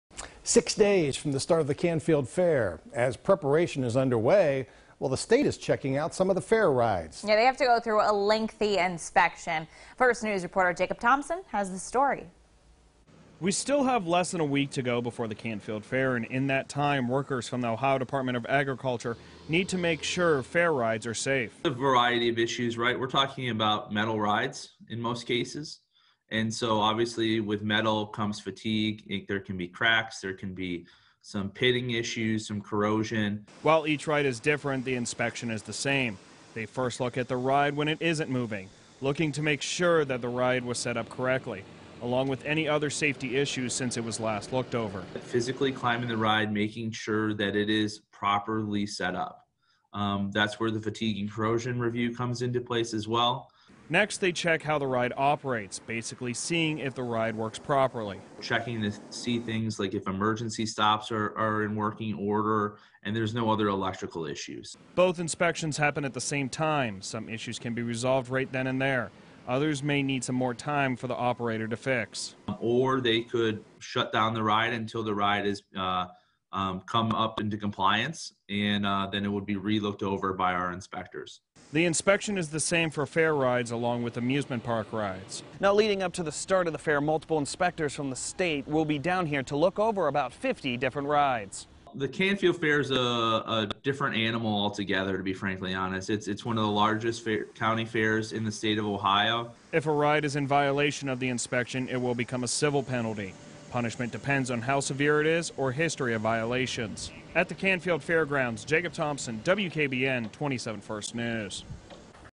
State ride inspectors in town making sure rides are up to code for Canfield Fair
Workers like David Miran from the Ohio Department of Agriculture are in town making sure the fair rides are safe.